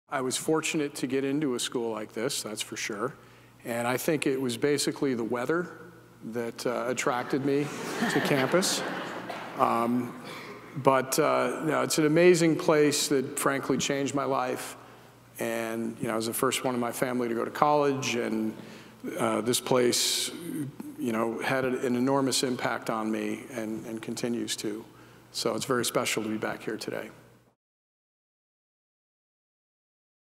Thomas Tull '92: Hamilton Changed My Life
Thomas Tull, a '92 Hamilton graduate, is the founder and former CEO of Legendary Entertainment where he produced more than 40  films including Godzilla, Jurassic World, the Dark Knight trilogy, The Town, Inception, and the Hangover franchise.

Now CEO of investment-holding company Tulco, Thomas talks about how Hamilton changed his life.

Hamilton, which has a need-blind financial aid policy, is one of the few U.S. colleges with an open curriculum, which means you’ll have the freedom to choose courses that reflect your interests.